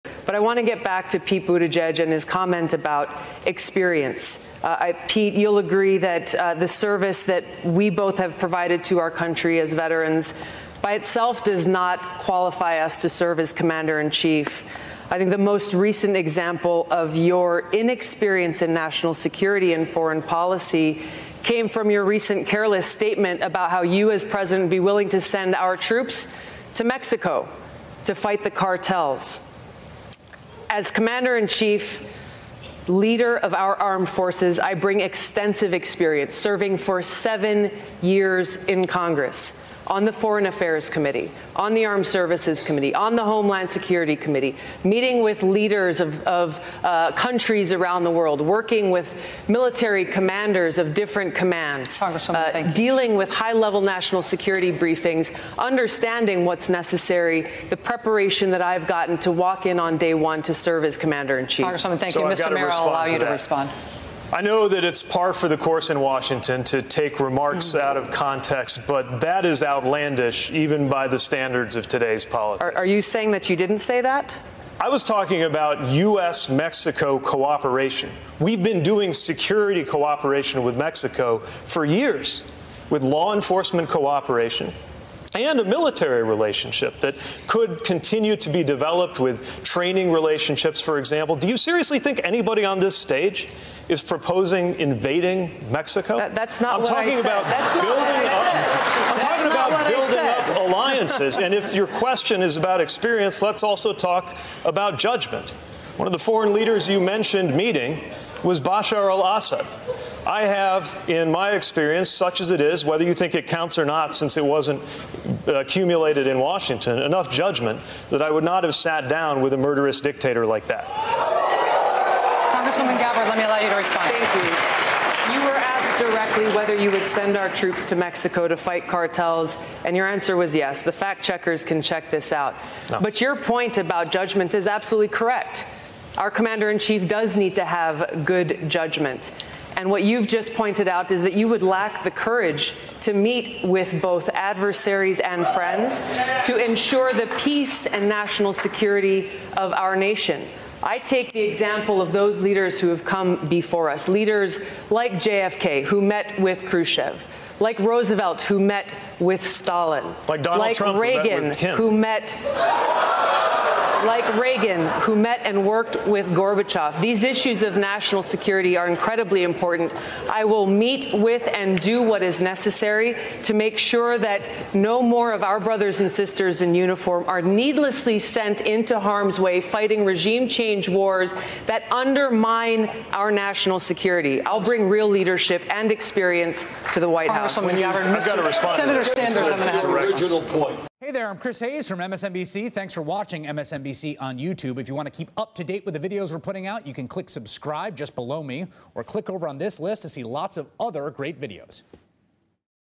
Pete Buttigieg And Tulsi Gabbard Call Each Other Out Over Inexperience | MSNBC
Pete Buttigieg and Tulsi Gabbard, both former veterans, take turns calling each other out over inexperience, and judgment. Aired on 11/20/19.